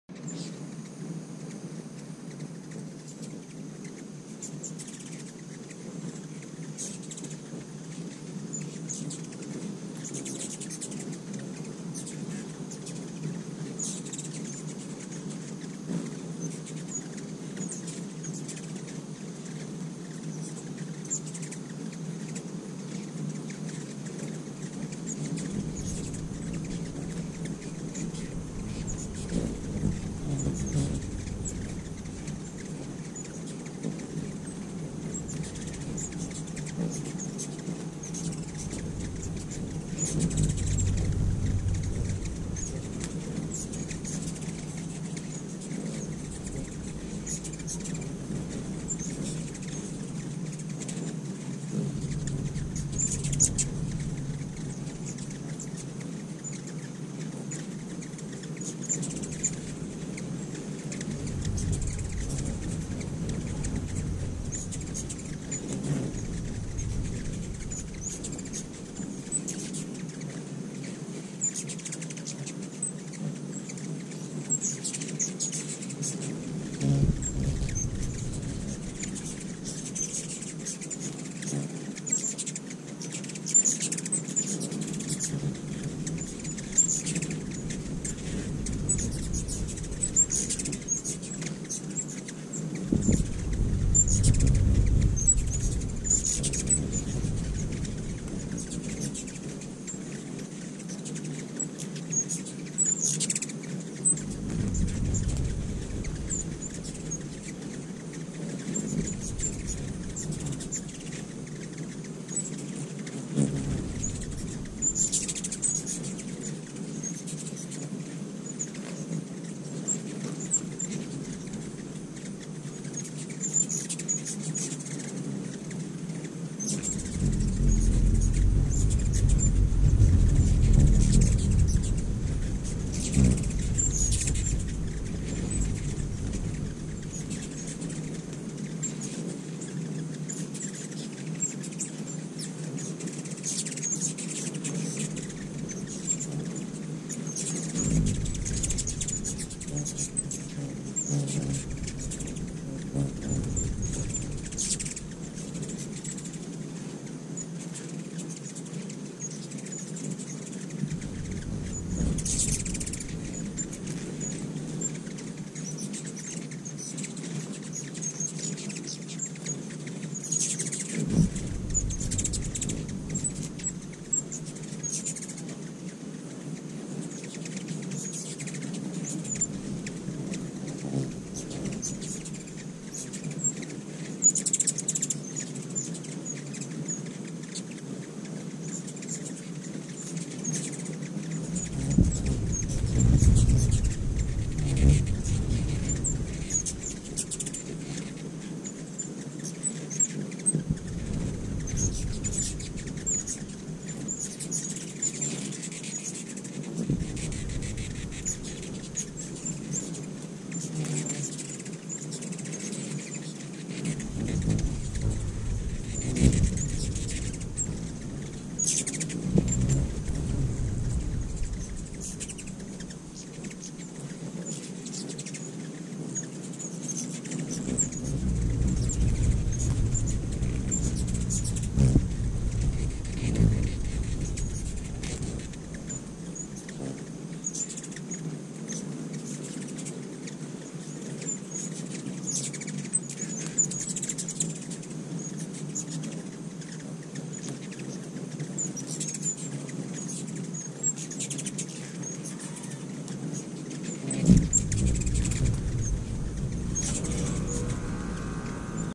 Ruby Throated Hummingbird Sights and Sounds
Its just a normal day at the feeders. 11 feeders in total. 1 cup sugar to 4 cups water (boiled)